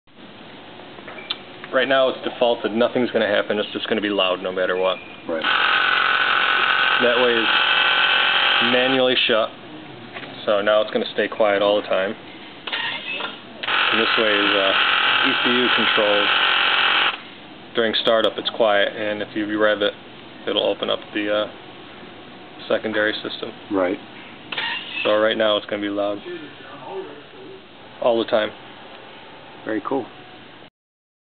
National Speed: 2007 Aston Martin Super Vantage - Exhaust Valve Control Switch Illustration...
This is a switching system we wired for the exhaust valves, so the end user can determine if they'd like the exhaust full-time open, full-time closed, or factory ECU controlled.